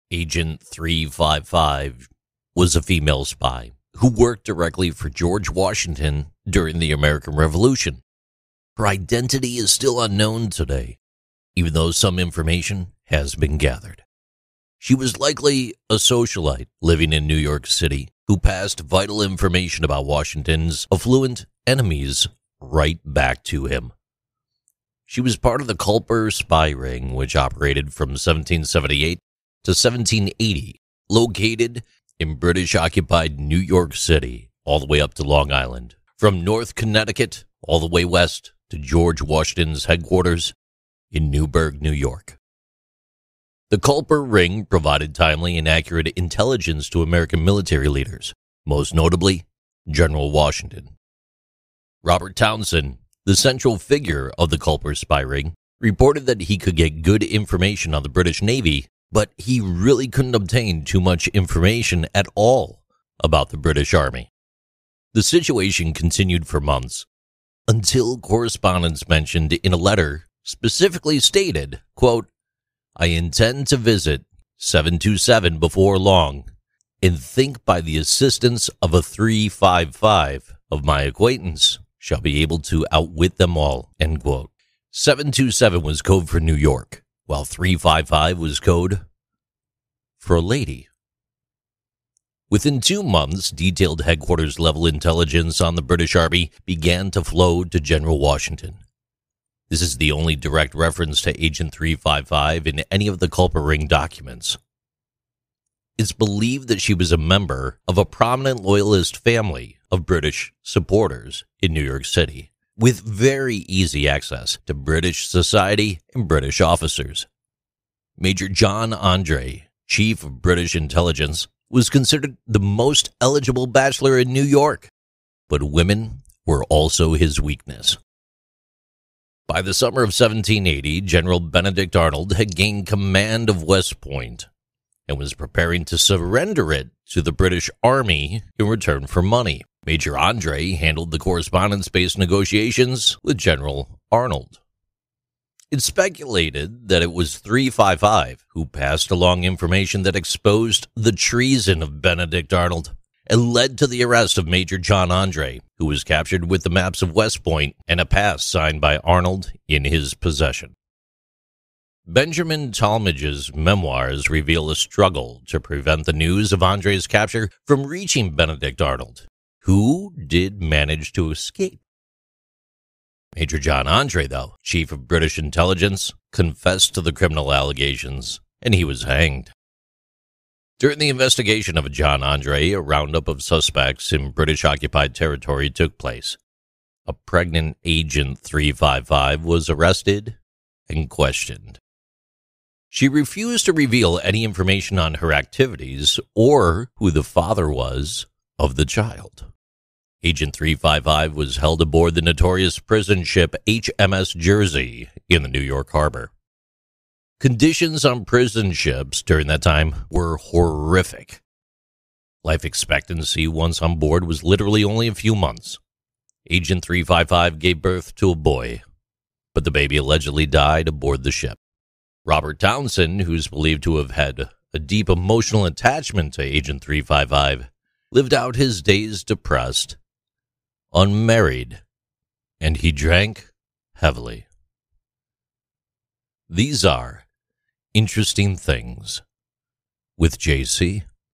160: "Agent 355"
interestingthings with JC #160: "Agent 355" was a female spy who worked directly for George Washington during the American Revolution, and was one of the first spies for the United States. Her real identity is still unknown to this day.